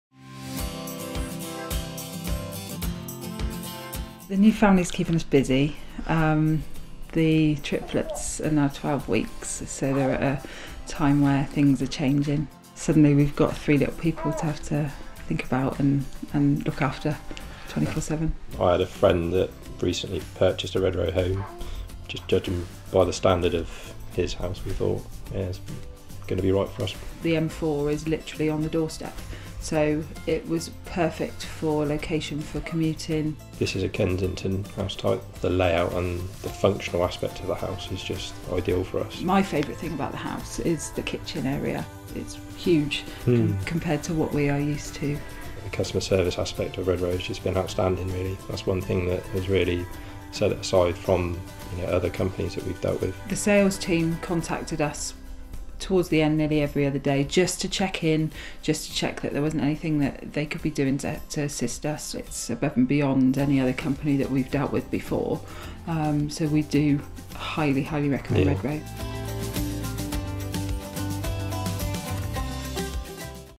Redrow New Homes - Badbury Park Customer Testimonial
Find out what our customers Matthew and Claire Midwinter have to say about their moving story